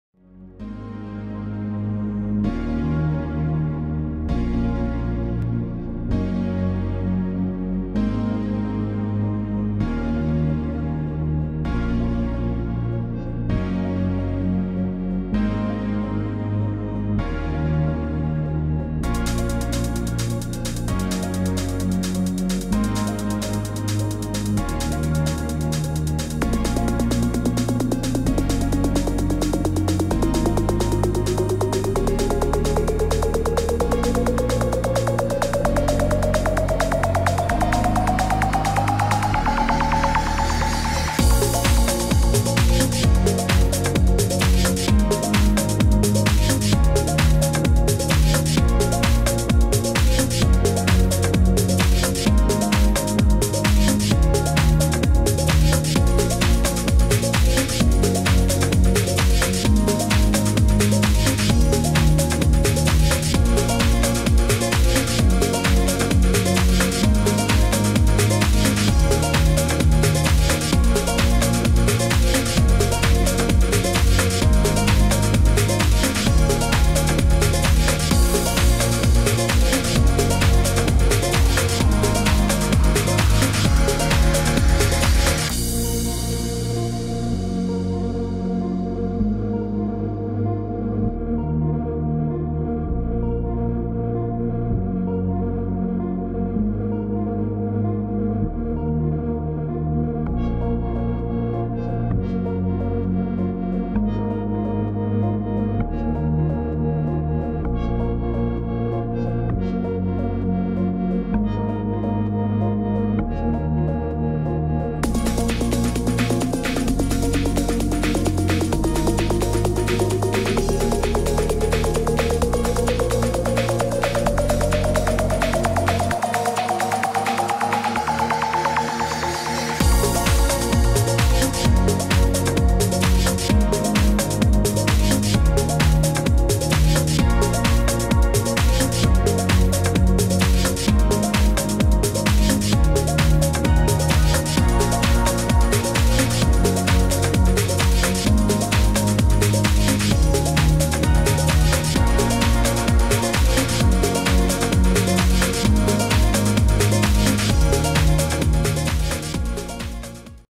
Żerowanie boleni na jeziorze / efektowne ataki boleni / boleń z jeziora
Tym razem podczas wypadu na okonie trafił się przyłów małego bolenia. Był to mój pierwszy złowiony boleń. Filmu z okoniami nie udało się nagrać bo nie chciały współpracować ale trafiłem w czas pięknego żerowania boleni z efektownymi wyskokami. Niestety na moją okoniową obrotówkę nie chciało już nic uderzyć.